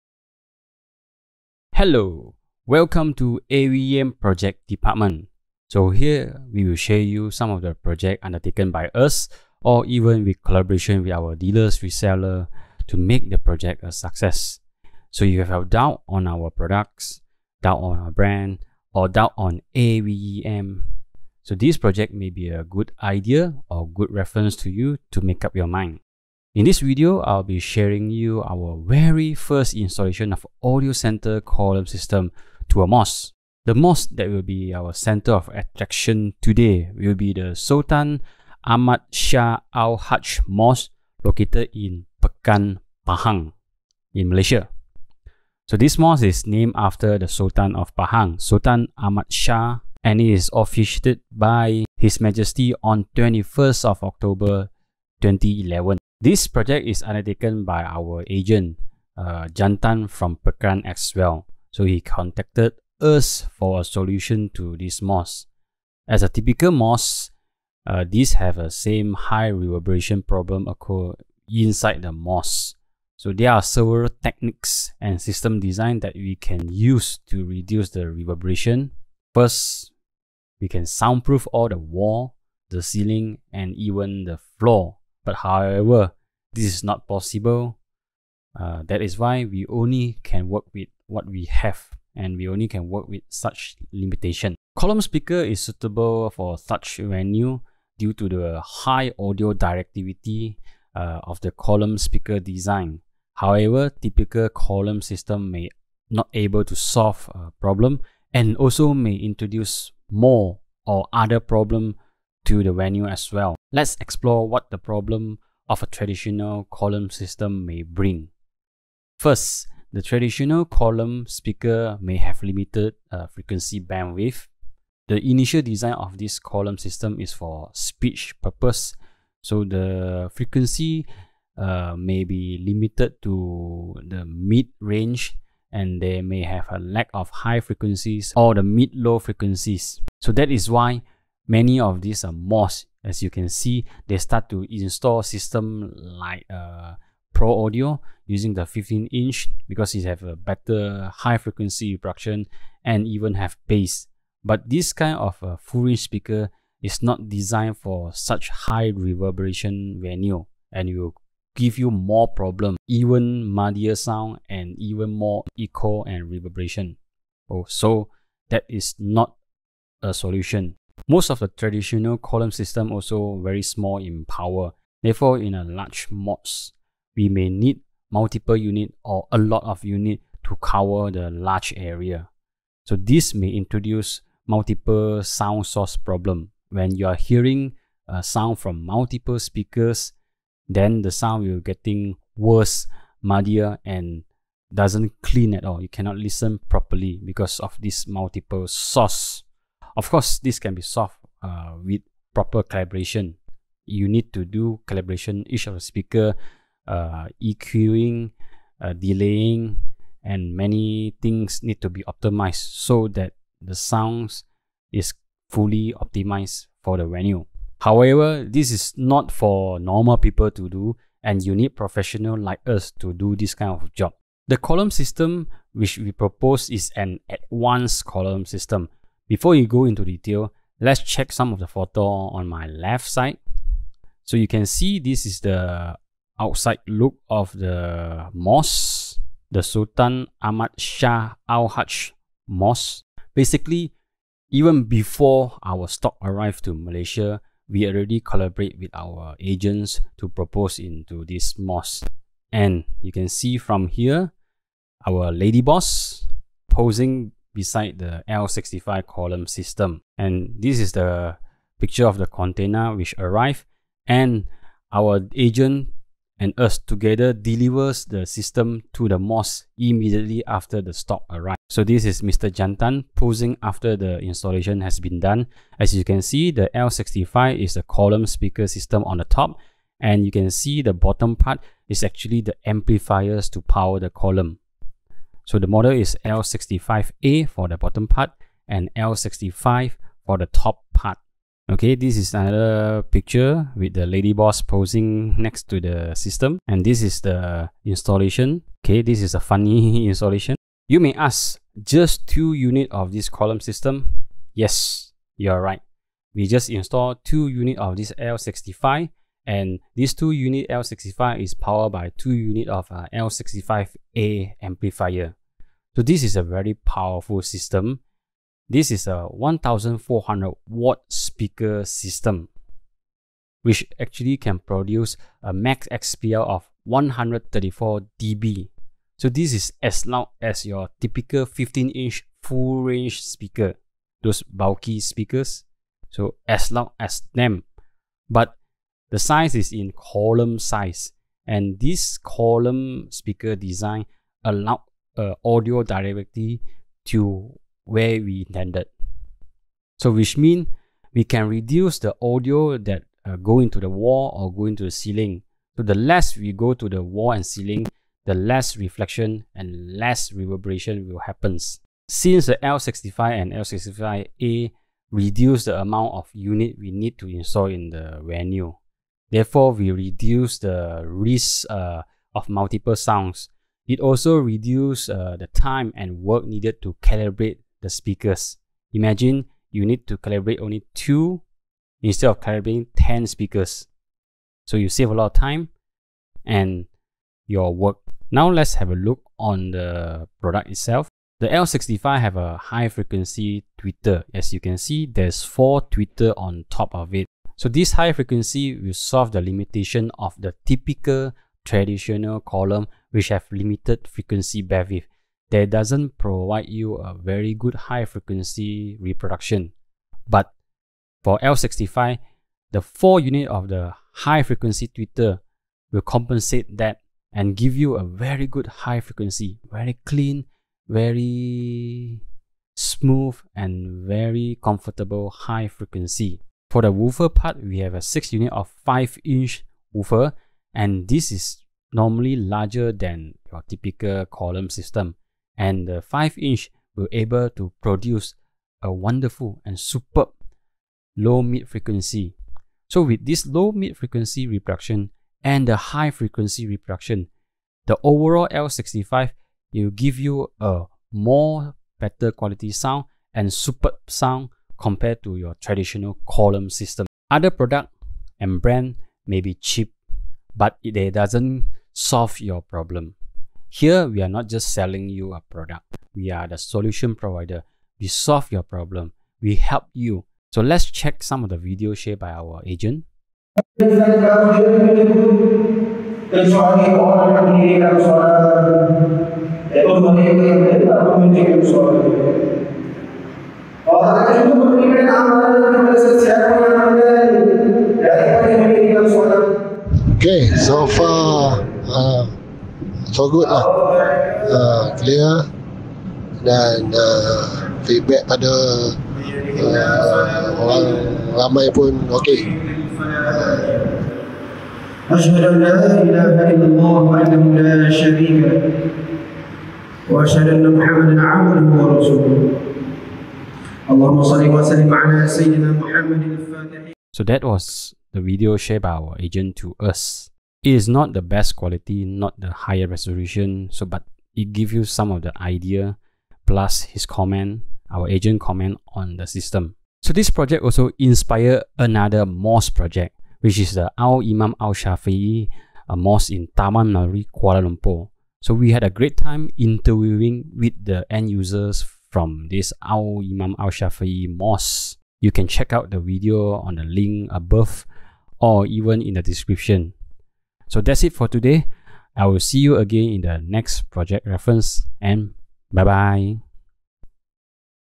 【AVEM PROJECT TALK 】PA System for Mosque -AUDIOCENTER install in Sultan Ahmad Shah Al Haj Mosque
This time, we install new high power and premium quality column array system into a mosque. This system solve many problem encountered by the highly reverberation venue such as mosque. Column system is a good solution for such venue but the traditional columns system maybe bring other problem into the venues. Audiocenter L65 + L65A system not only utilize column technology, it also revolutionize the column technology. Not only it has the beam shaping advantage of a column, it also does not bring the additional problem that brought by traditional column system.